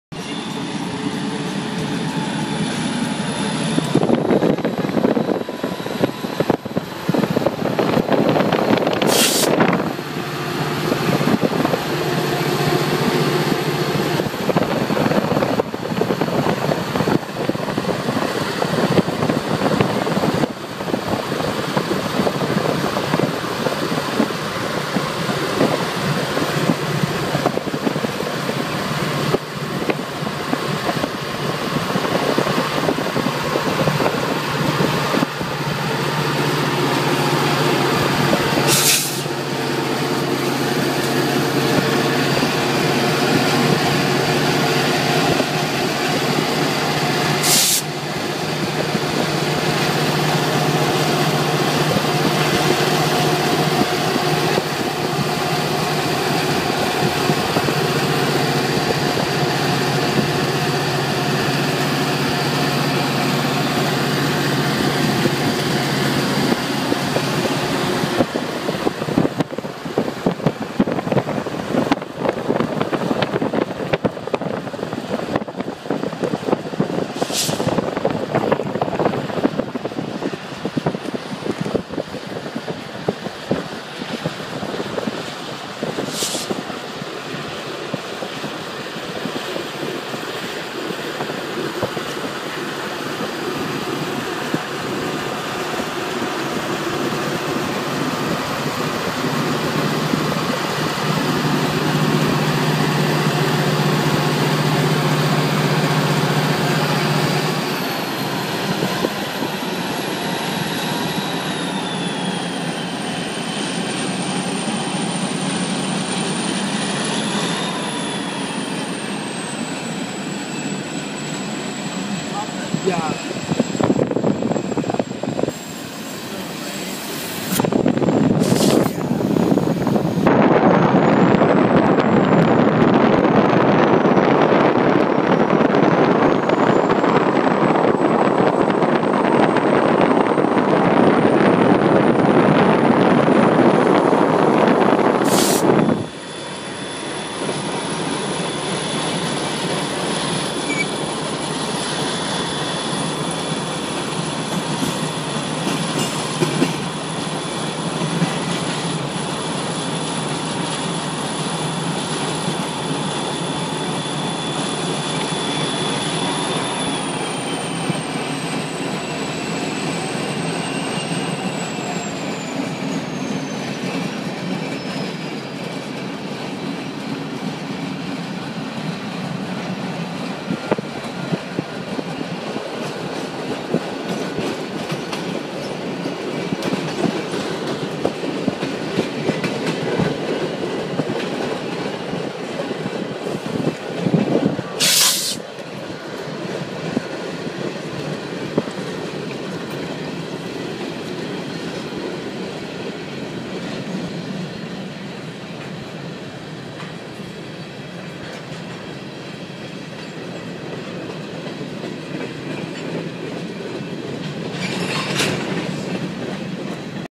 BNSF 2721 LHF 9-26-13
BNSF 2721 gp39-2 running long hood forword going to Longview yard with a string of grain cars on BNSF seattle sub watch in 1080P